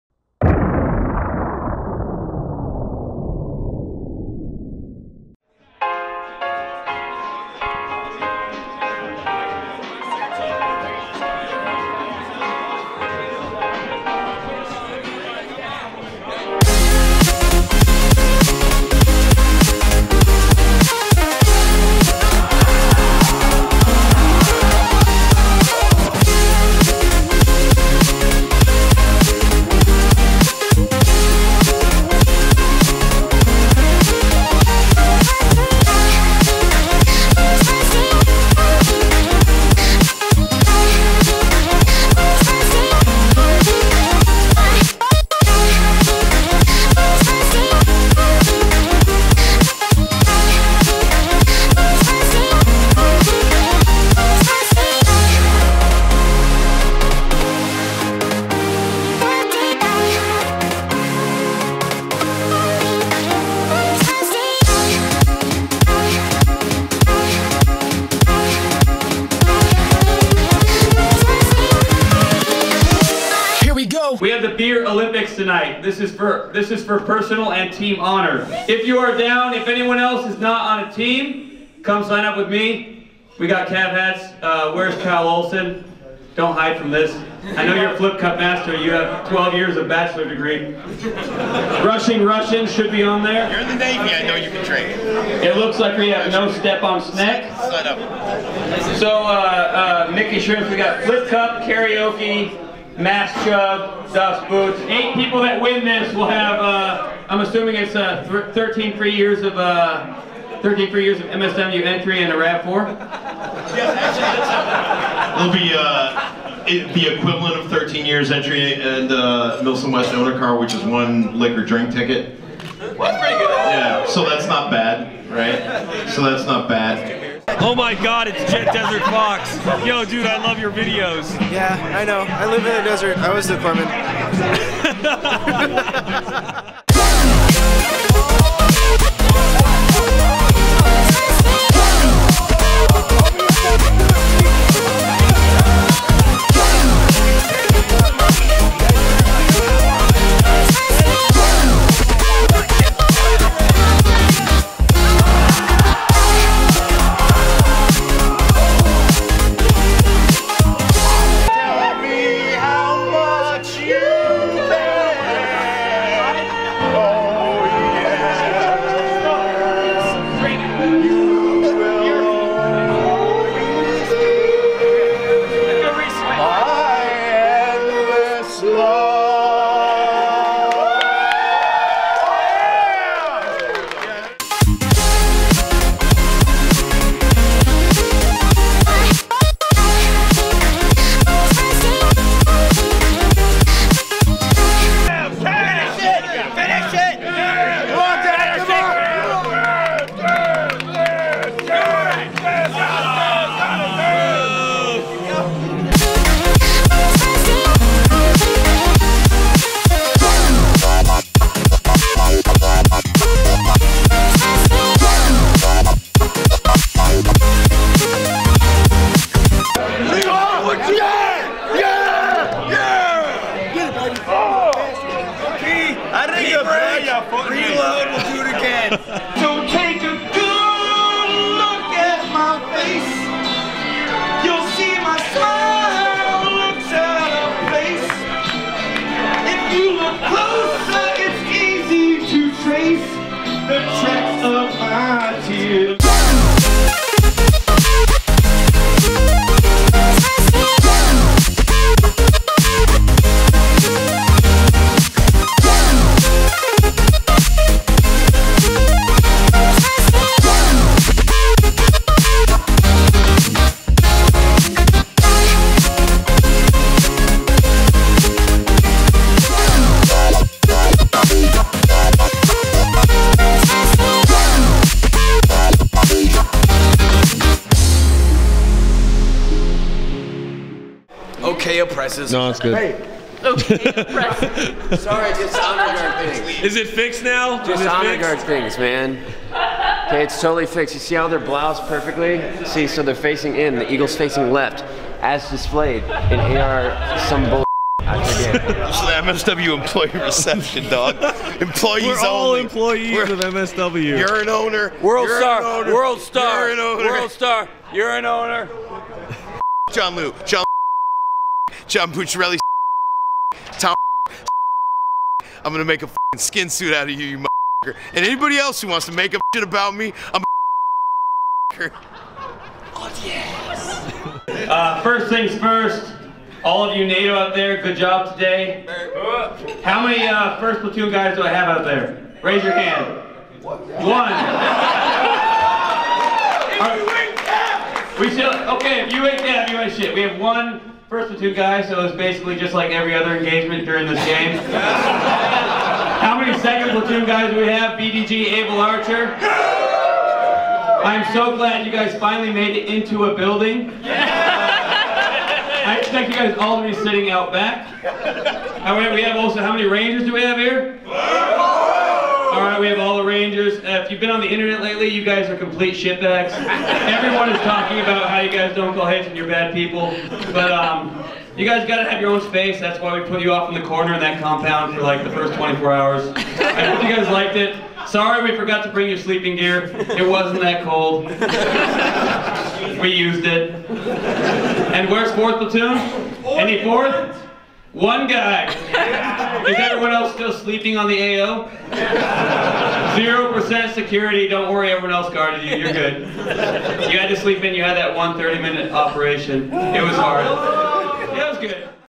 Beer Olympics: The Party After Grozny (Milsim West Official After Party)
Bringing Russians, Militia, and NATO together in the name of having fun: the official Milsim West after party. Behind the scenes for the party after Milsim West: Seize Grozny. The official after party included dinner and 2 drink tickets (for those over 21), plus an evening of telling airsoft war stories and bonding with your fellow teammates/"enemies". This gives airsofters the chance to hang out with the Milsim West cadre/veterans and allows everyone to connect outside of airsoft in general. It was a lot of fun and I hope that this continues to happen after future airsoft events.

THIS IS AN AIRSOFT GAME. THIS IS A PRETEND LIVE ACTION ROLE PLAYING GAME. THIS IS AIRSOFT.

Follow Jet DesertFox and I on Twitch: desertfoxunicorn